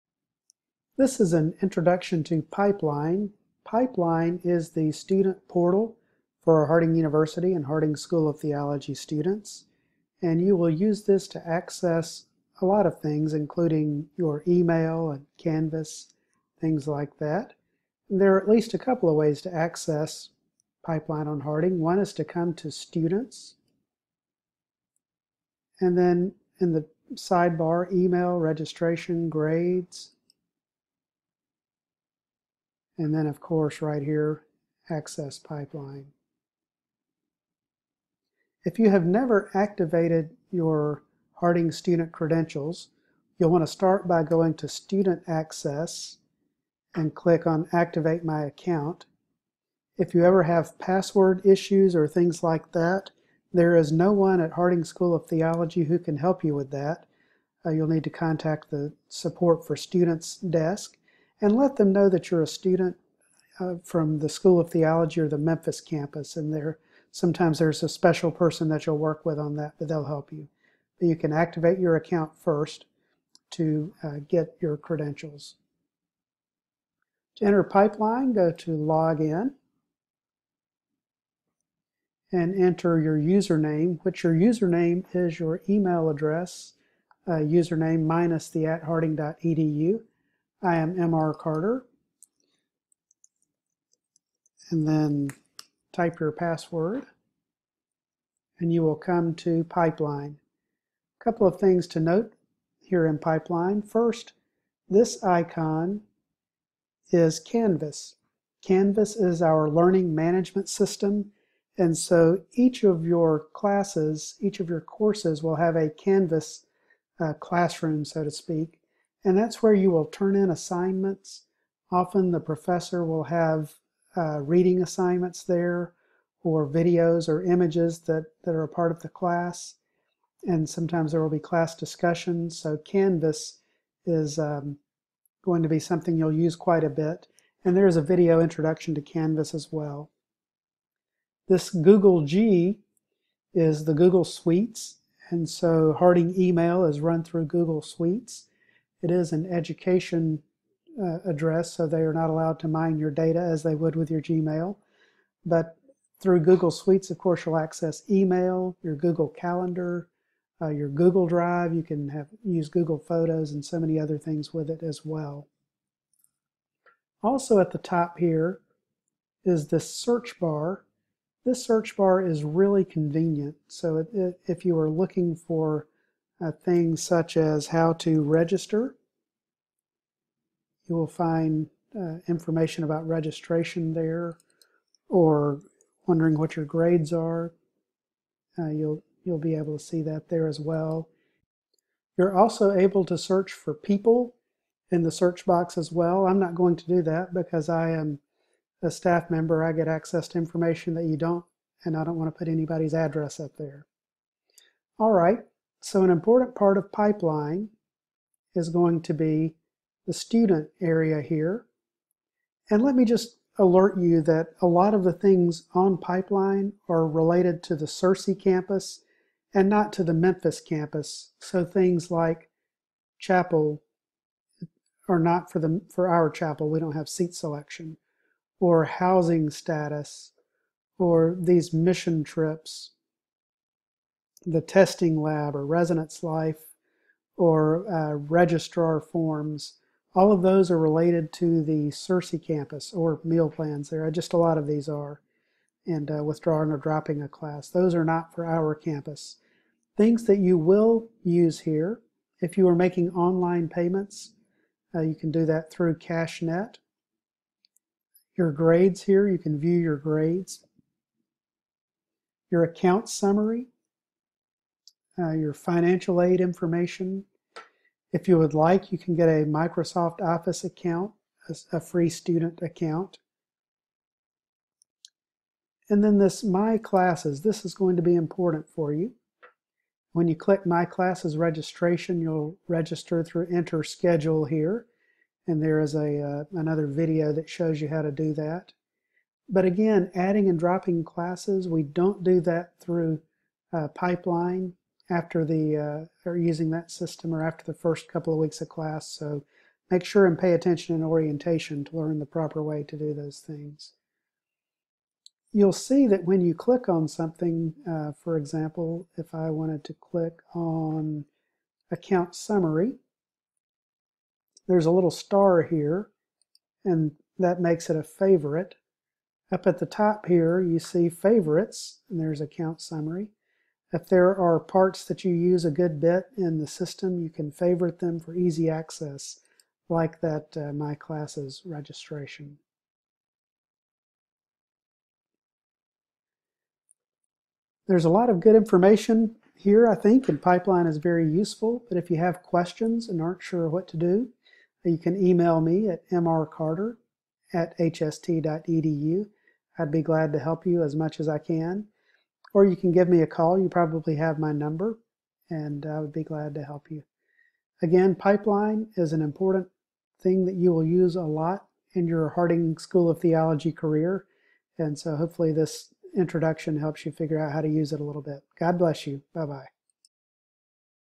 Introduction to Pipeline
Pipeline is the student portal to many online aspects of Harding University. This tutorial will show you how to access Pipeline and introduce you to a few of the features you will use regularly.